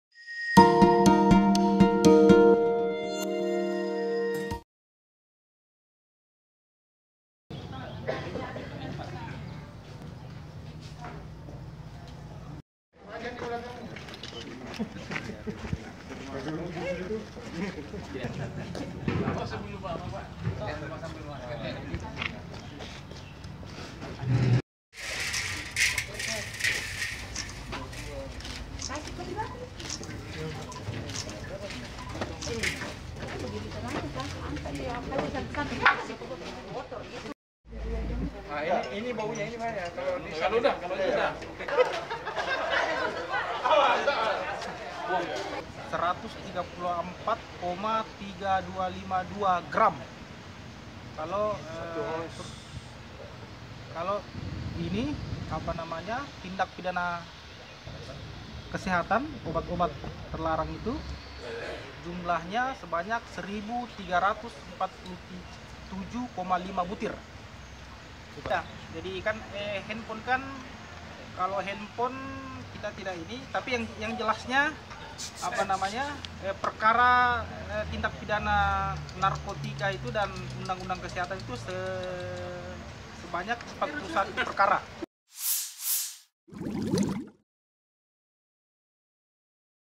Inkracht, Kejari Barru Musnahkan Barang Bukti Sabu dan Obat Terlarang
SULSELSATU.com, BARRU — Kejaksaan Negeri Barru melakukan pemusnahan barang bukti narkotika dan obat terlarang di halaman kantor Kejari Barru, Selasa (17/7/2018).

Kepala Kejari (Kajari) Barru, Paian Tumanggor mengatakan, pemusnahan barang bukti tersebut atas dasar adanya putusan hakim yang telah berkekuatan hukum tetap (inkracht).

Videographer: Asriadi Rijal
Video Editor: Andi Hermanto
Editor: Hendra Wijaya